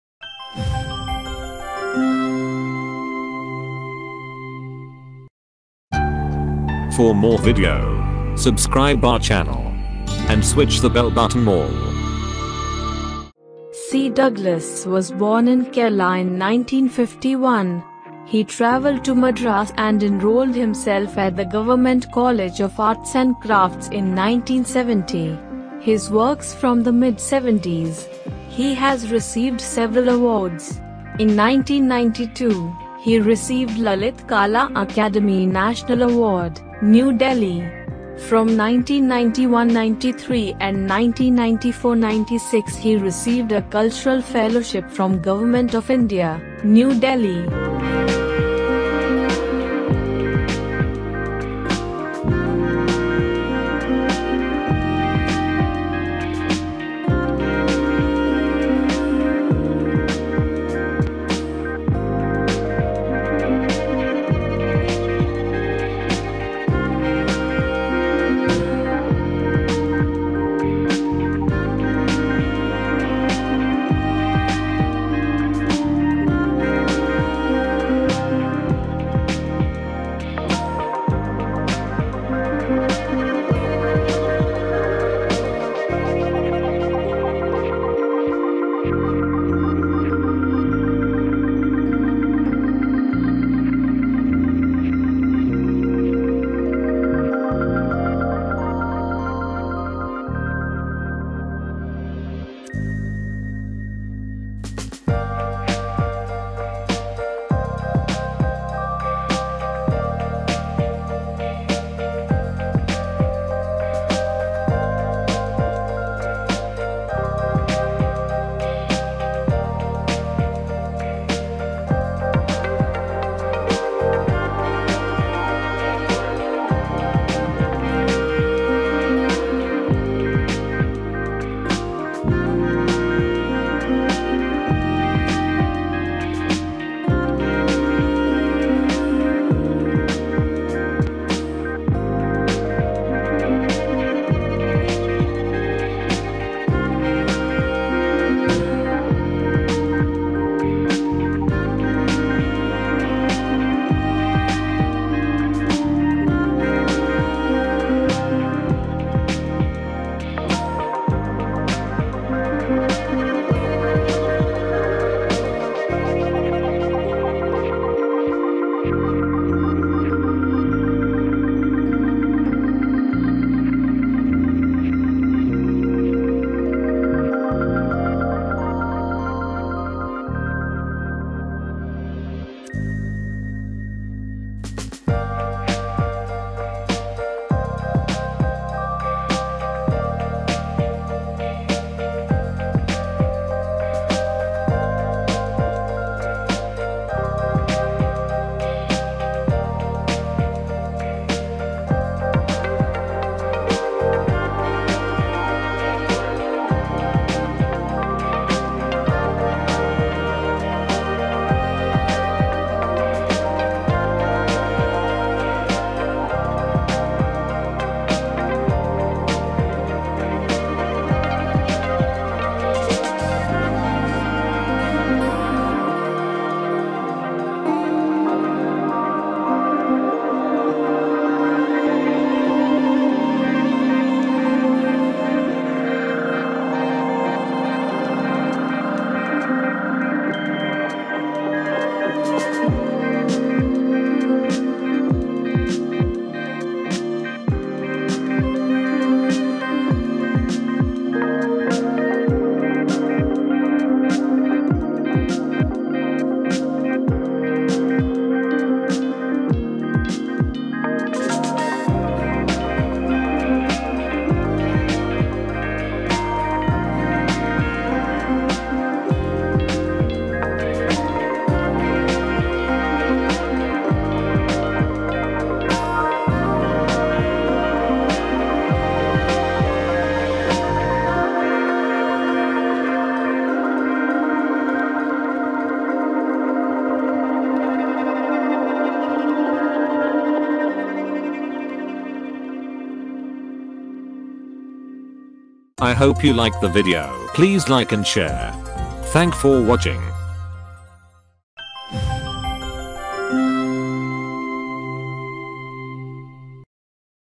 Indian Kerala Artist C. Douglas, Collection of Painting
C. Douglas was born in Kerala in 1951. He travelled to Madras and enrolled himself at the Government College of Arts and Crafts in 1970. His works from the mid 70’s. He has received several awards.  In 1992, he received Lalit Kala Akademi National Award,  New Delhi. From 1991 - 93 and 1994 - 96 he received a Cultural Fellowship from Government of India, New Delhi.